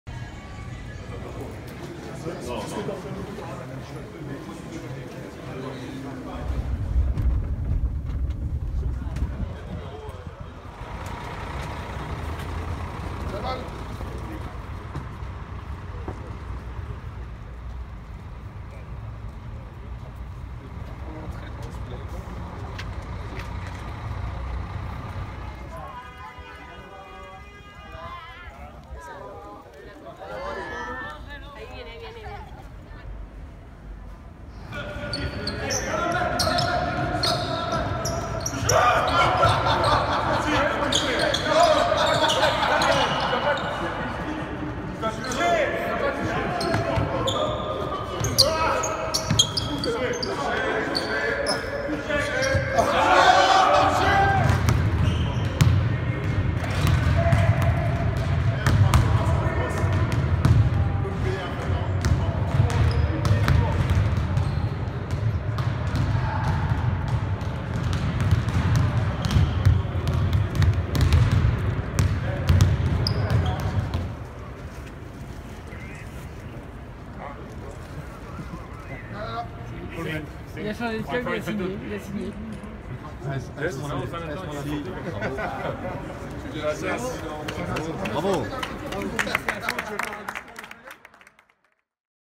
MissionRio - 28 juillet - Premier jour à Cordoba
Vivez avec les Bleus leur premier journée à Cordoba (Argentine) en vue du Tournoi "Super 4" ce week-end #‎MissionRio‬